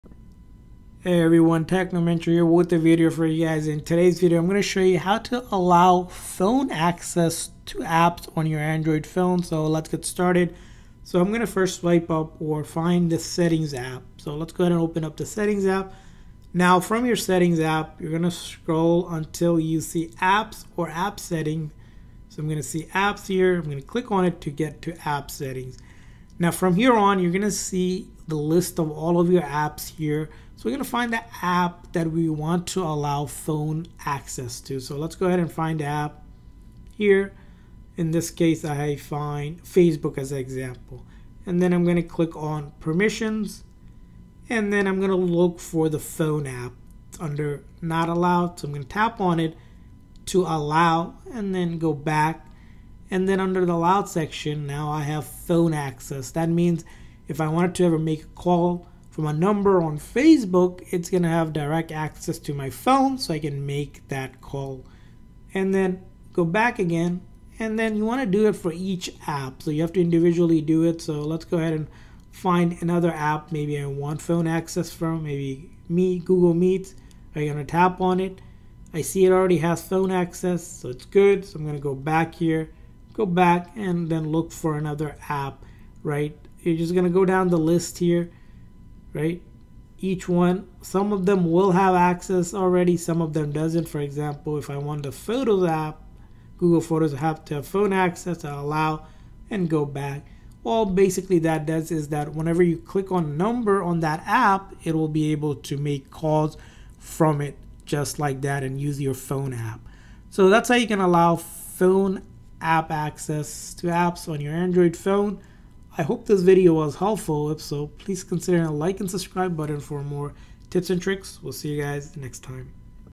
How To Allow Phone Access To Apps On Android Phone
Here's how to give phone access to apps on your Samsung or Android phone!